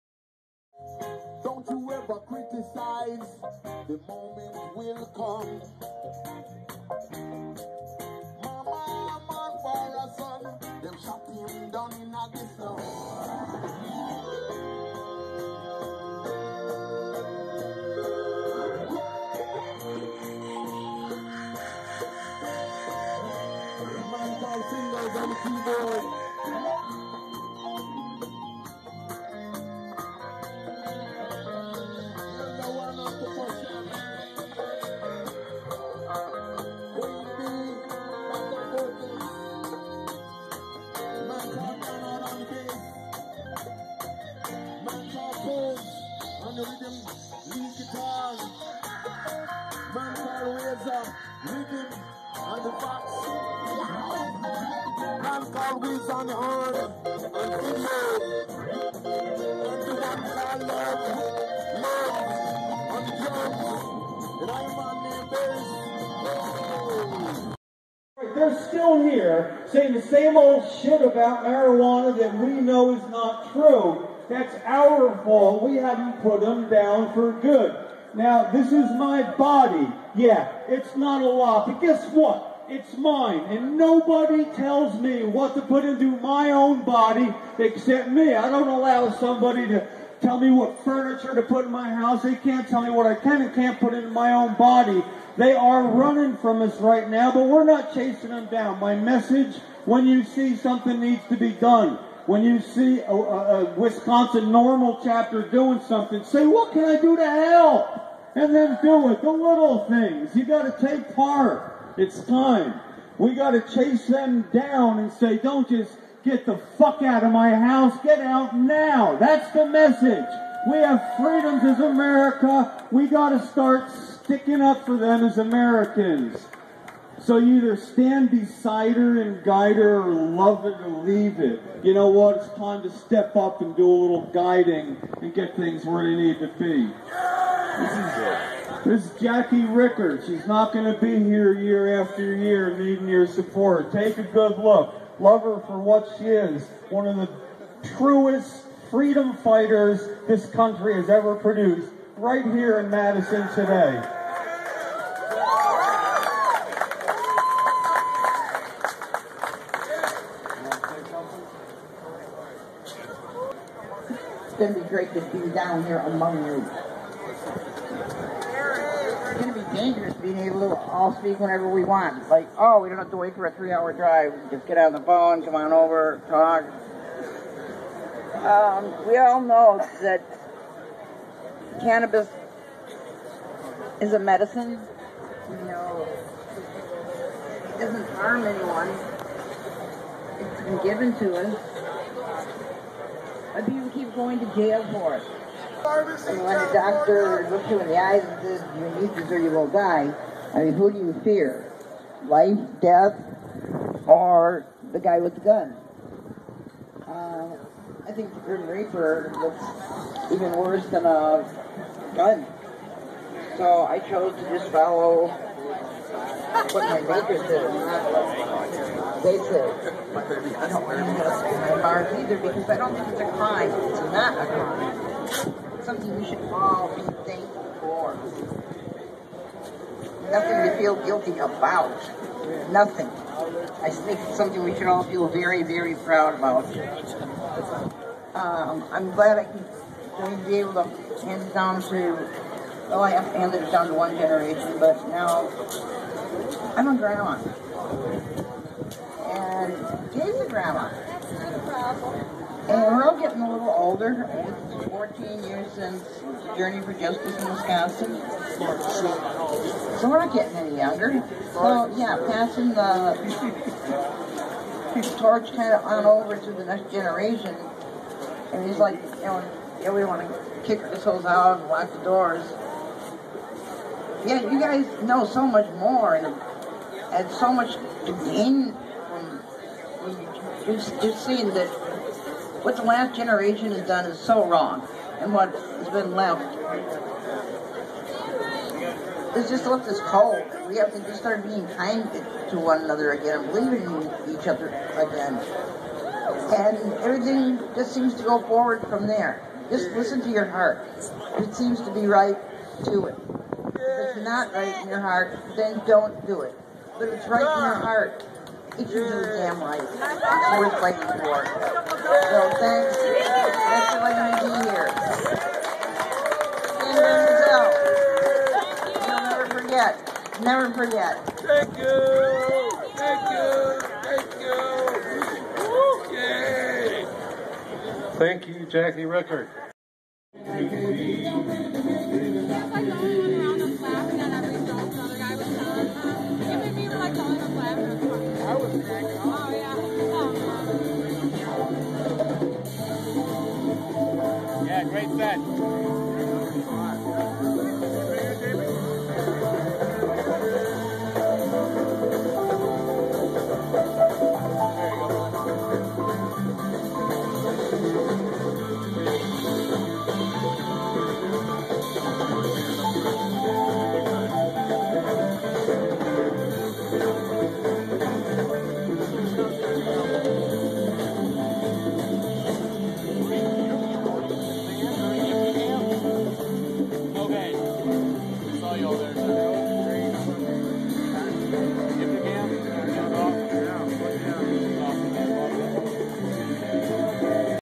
NORML 41st Harvest Fest w Julie Ricker
Heres footage from NORML's 41st Harvest Fest in Madison, WI. I love
Madison!!!